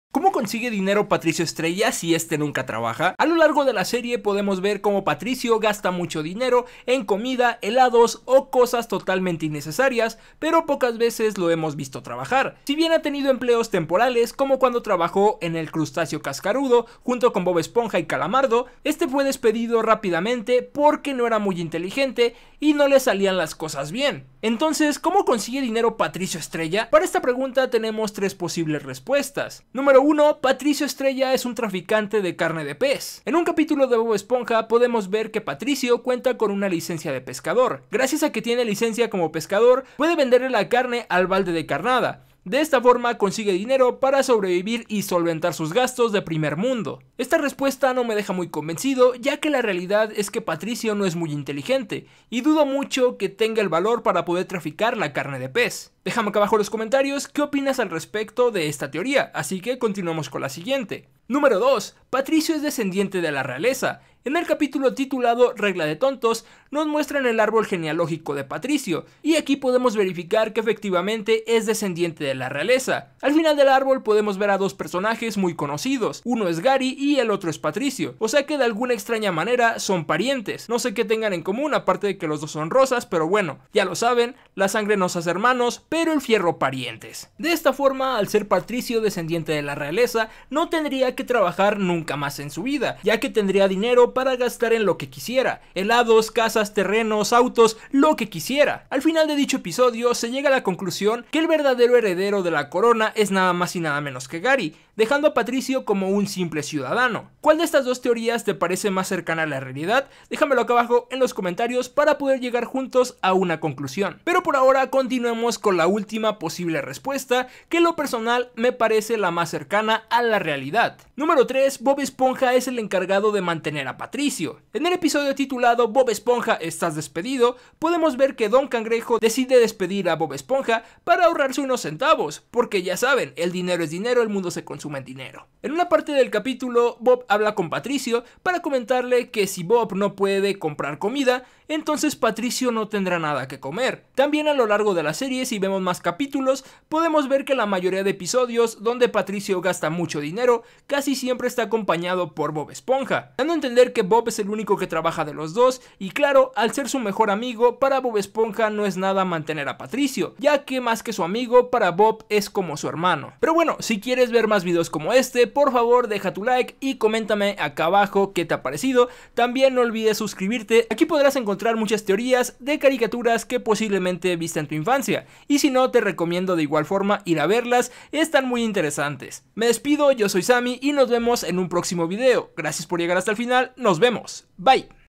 ¿Cómo Consigue Dinero Patricio Si No Trabaja?
-¿Como Consigue Dinero Patricio Sin Trabajar? Entonces has llegado al lugar indicado..

🤜🤛Unete a nuestra comunidad en discord! :D

▼ CONTACTO ▼

¿Te gustaría saber porque Patricio tiene dinero si no trabaja?. En mi vídeo hablaremos de bob esponja y sobre todo de patricio.